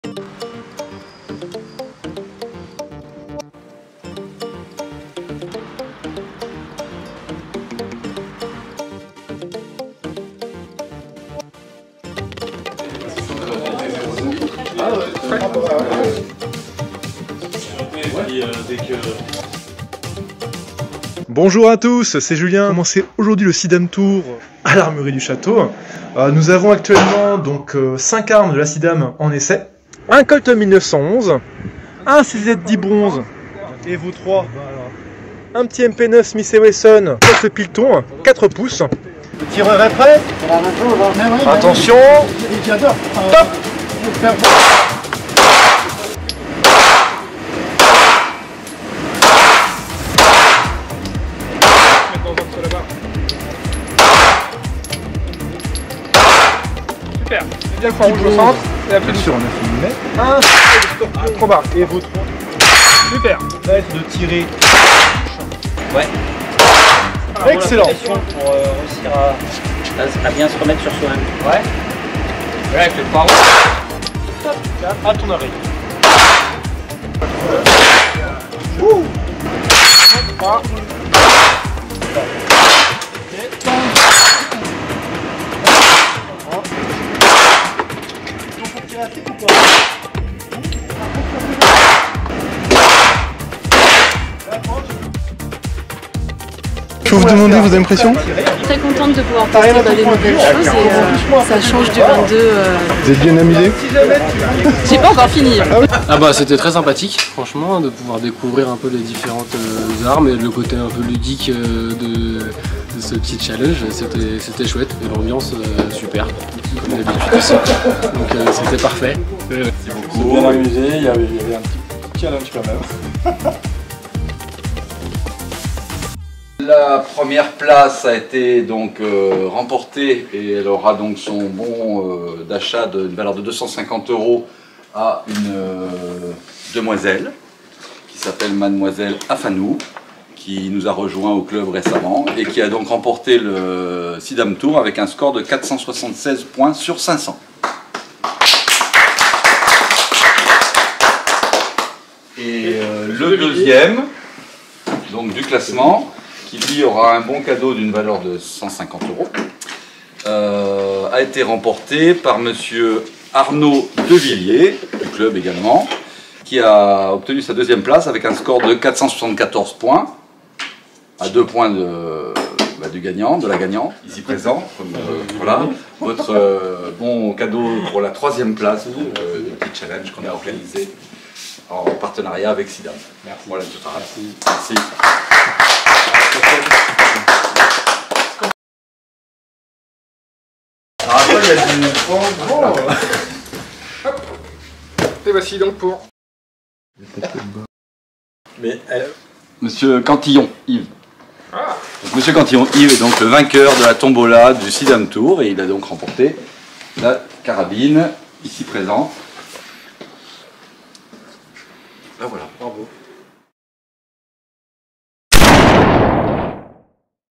Revivez l'expérience Sidam tour 2023 à l'Armurerie du Château, stand de tir 77
Revivez l'incroyable aventure du Sidam Tour 2023 à l'Armurerie du Château,  situé dans le département 77. Cet événement unique a transporté les passionnés d'armes à feu sur notre stand de tir.
Lors de cet événement mémorable, les participants ont eu l'occasion de découvrir une sélection exceptionnelle d'armes.
Des pistolets aux fusils d'assaut en passant par les fusils de précision, chaque arme a été soigneusement choisie pour offrir une expérience de tir inoubliable.
Bravo aux gagnants et un grand merci à tous les participants et à notre partenaire Sidam !